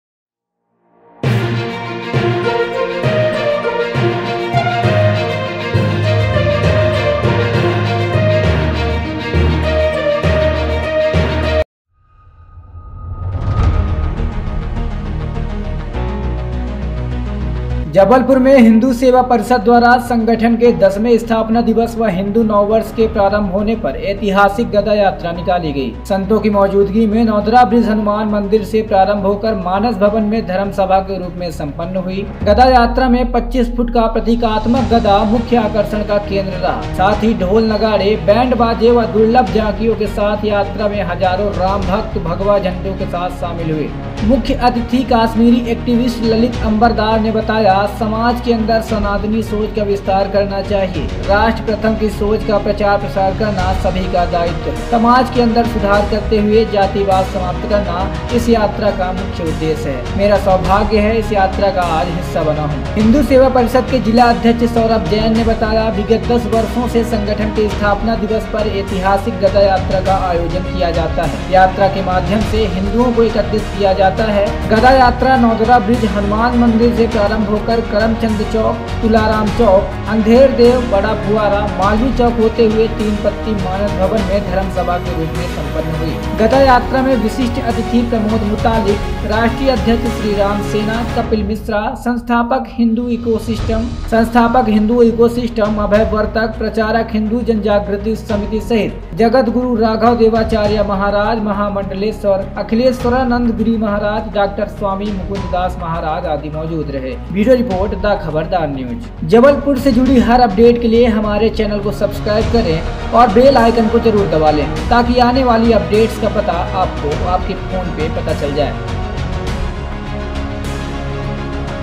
हजारों की संख्या में रामभक्तों की यात्रा:जबलपुर में हिंदू सेवा परिषद ने निकाली गदा यात्रा
हजारों की संख्या में रामभक्तों की यात्रा:जबलपुर में हिंदू सेवा परिषद ने निकाली गदा यात्रा, कश्मीरी एक्टिविस्ट ललित अंबारदार ने कहा राष्ट्र प्रथम की सोच के साथ जातिवाद समाप्त करना उद्देश्य
हिंदू सेवा परिषद द्वारा संगठन के 10 वें स्थापना दिवस व हिंदू नव वर्ष के प्रारंभ होने पर ऐतिहासिक गदा यात्रा निकाली गई। संतों की मौजूदगी में नौदरा ब्रिज हनुमान मंदिर से प्रारंभ होकर मानस भवन में धर्म सभा के रूप में संपन्न हुई। गदा यात्रा में 25 फुट का प्रतीकात्मक गदा मुख्य आकर्षण का केंद्र रहा। साथ ही ढोल नगाड़े, बैंड बाजे व दुर्लभ झांकियों के साथ यात्रा में हजारों राम भक्त भगवा झंडो के साथ शामिल हुए।
मुख्य अतिथि कश्मीरी एक्टिविस्ट ललित अंबरदार ने बताया समाज के अंदर सनातनी सोच का विस्तार करना चाहिए। राष्ट्र प्रथम की सोच का प्रचार-प्रसार करना सभी का दायित्व हैं। समाज के अंदर सुधार करते हुए जातिवाद समाप्त करना इस यात्रा का मुख्य उद्देश्य है। मेरा सौभाग्य है इस यात्रा का आज हिस्सा बना हूं।
हिंदुओं को एकत्रित करने की सोच

हिंदू सेवा परिषद के जिला अध्यक्ष सौरभ जैन ने बताया विगत 10 वर्षों से संगठन के स्थापना दिवस पर ऐतिहासिक गदा यात्रा का आयोजन किया जाता है। यात्रा के माध्यम से हिंदुओं को एकत्रित किया जाता हैं और उनके अंदर देश प्रेम की भावना जागृत की जाती है। गदा यात्रा नौदरा ब्रिज, हनुमान मंदिर से प्रारंभ होकर करमचंद चौक, तुलाराम चौक, अंधेरदेव, बड़ा फुआरा, मालवीय चौक होते हुए तीन पत्ती मानस भवन में धर्म सभा के रूप में संपन्न हुई।

ये हुए शामिल

गदा यात्रा में विशिष्ट अतिथि प्रमोद मुतालिक, राष्ट्रीय अध्यक्ष श्रीराम सेना, कपिल मिश्रा, संस्थापक हिंदू इकोसिस्टम, अभय वर्तक, प्रचारक हिंदू जनजागृति समिति सहित जगद्गुरु राघव देवाचार्य महाराज, महामंडलेश्वर अखिलेश्वरनंद गिरि महाराज, डॉक्टर स्वामी मुकुंद दास महाराज आदि मौजूद रहे।